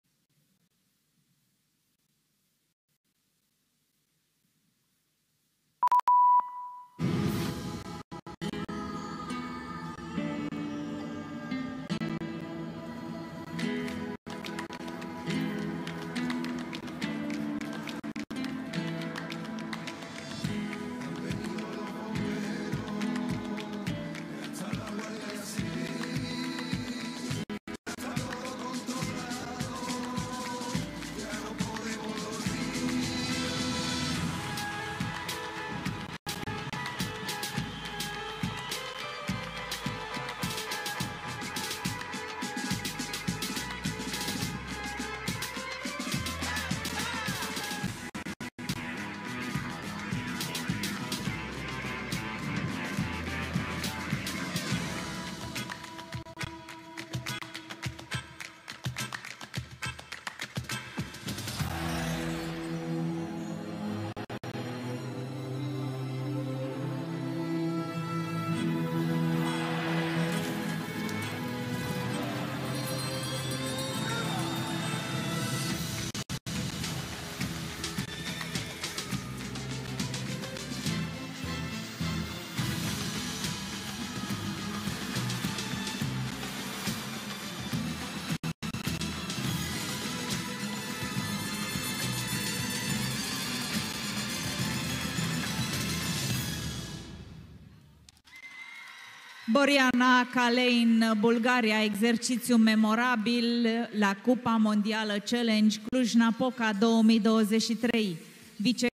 Boryana Kaleyn (BUL) Clubs Qualification Rhythmic Gymnastics World Challenge Cup Cluj-Napoca 2023
Score 34.2
Video from Rhythmic Gymnastics World Challenge Cup 2023 YouTube Channel. Music rights to owners.